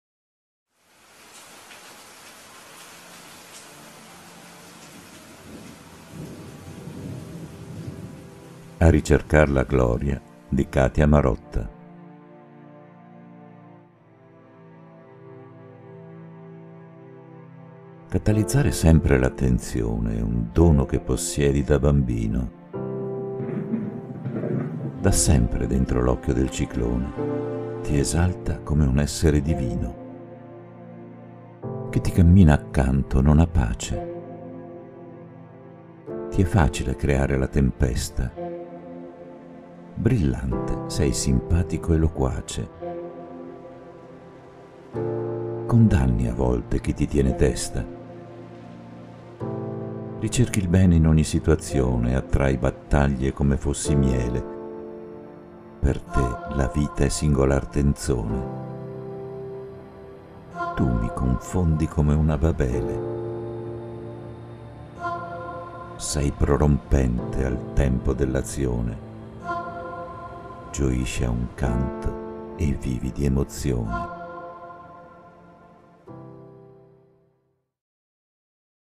A RICERCAR LA GLORIA Poesia di Katya Marotta dedicata al figlio Fabrizio Cacciola, autore del video
Catalizzare sempre l'attenzione
E' un dono che possiedi da bambino,
Da sempre dentro l'occhio del ciclone,
Ti esalta come un essere divino.
Chi ti cammina accanto non ha pace,
Ti è facile creare la tempesta,
Brillante, sei simpatico e loquace,
Condanni a volte chi ti tiene testa.
Ricerchi il bene in ogni situazione,
Attrai battaglie come fossi miele,
Per te la vita è singolar tenzone.
Tu mi confondi come una Babele,
Sei prorompente al tempo dell'azione,
Gioisci a un canto e vivi di emozione.